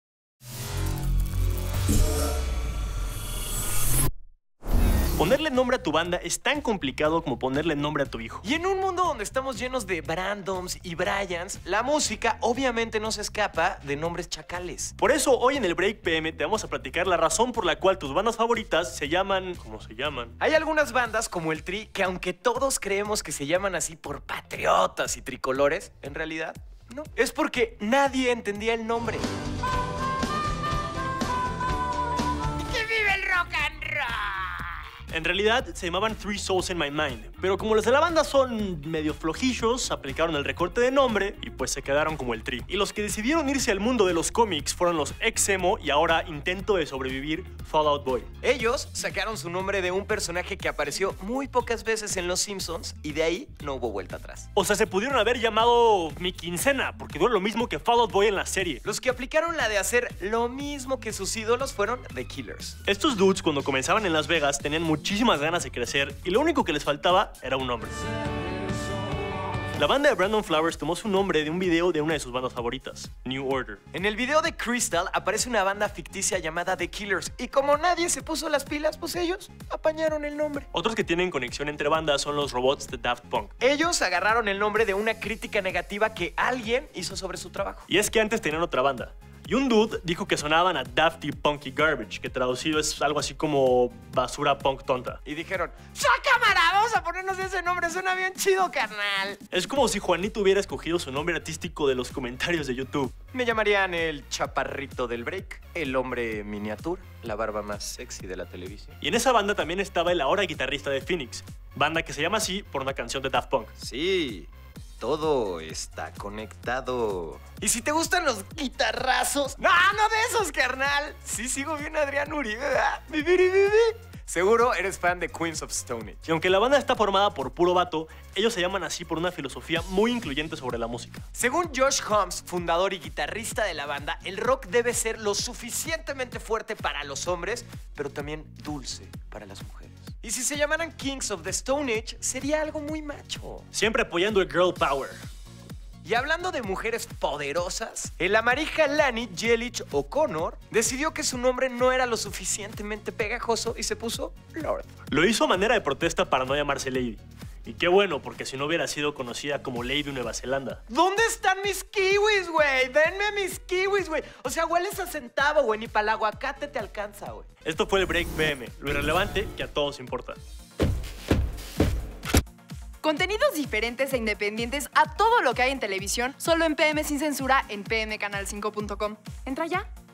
El Break PM - ¿De dónde viene el nombre de tu banda favorita?
Si crees que detrás del nombre de tus bandas favoritas hay un significado muy profundo, ¡no te equivoques! ElBreakPM te trae la historia de por qué se llaman así y no es precisamente por lo que estás pensando.